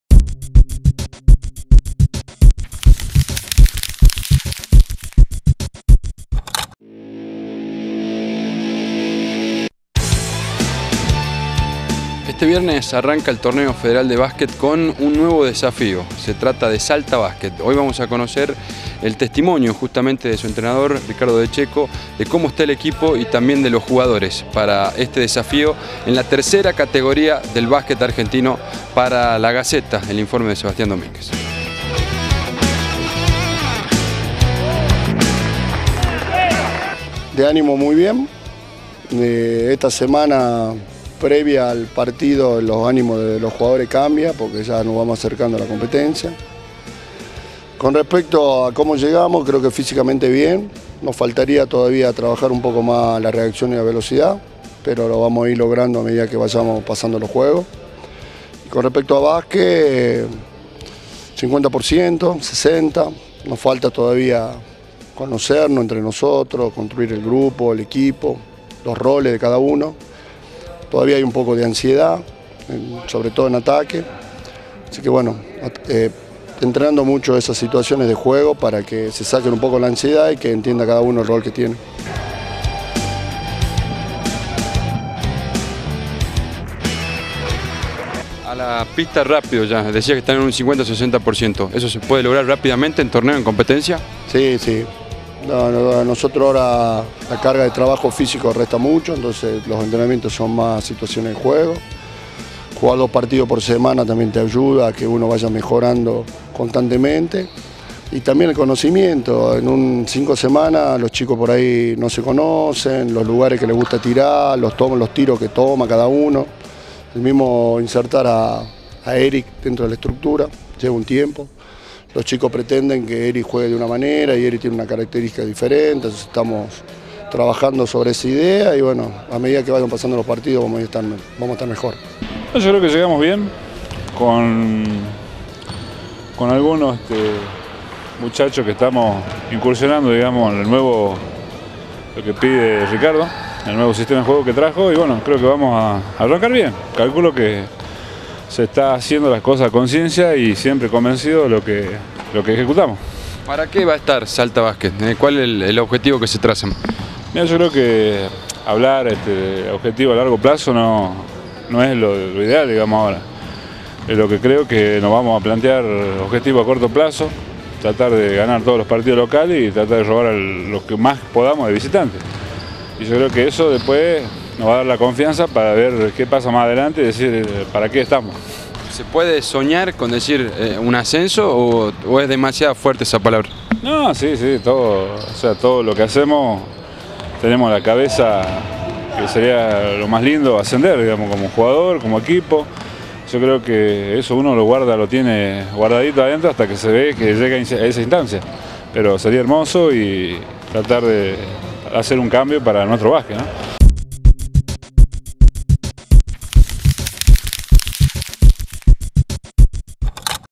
La selección Salta Basket se prepara para su debut
El seleccionado Salta Basket jugará mañana desde las 22 en Jujuy. Debutará el equipo que representará a la ciudad en la competencia de la tercera categoría nacional. Mirá el informe de LA GACETA.